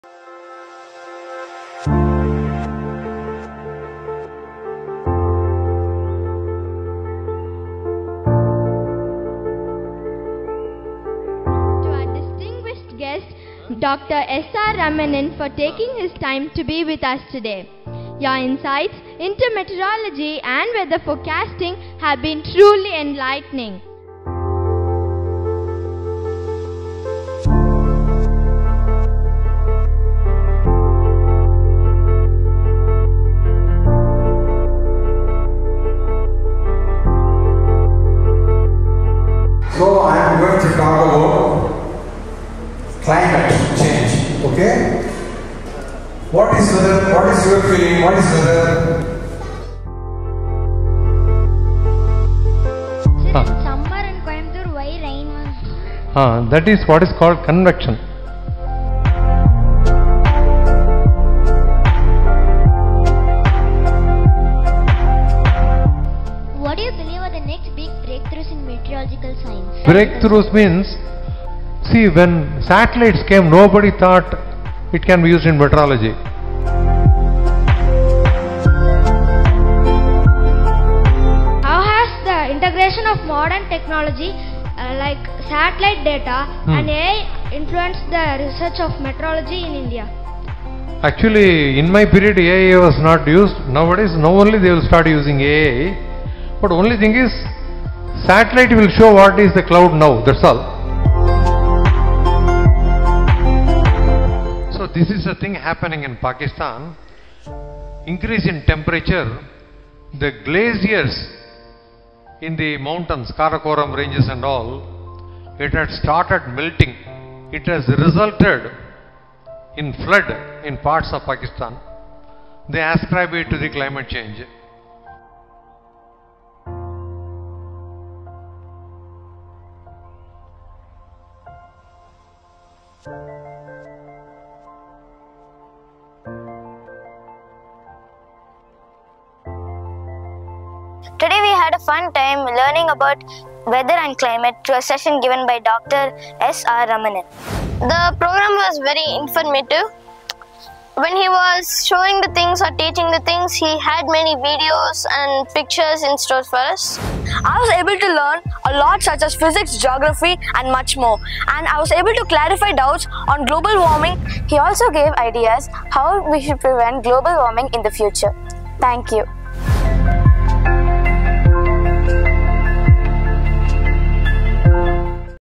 Dr. S. R. Ramanan | A detailed overview of climate change and its effects
“In this insightful session, renowned meteorologist Dr. S.R. Ramanan shares his expertise on the climate crisis, its causes, impacts, and the necessary actions for a sustainable future. 🌱

Watch as Dr. Ramanan breaks down complex climate science into simple, relatable concepts, offering our students a unique opportunity to learn from an expert in the field. From the melting glaciers in Pakistan to global weather patterns, this talk covers it all. 🌦

Highlights of the session include:

A detailed overview of climate change and its effects 🌍
Real-world examples of environmental impact 🏔️
Engaging Q&A with students, sparking curiosity and thoughtful discussion 🗣️
Practical solutions for a sustainable future 🌿
Let’s come together to take action for our planet! Watch, learn, and be inspired to make a difference.”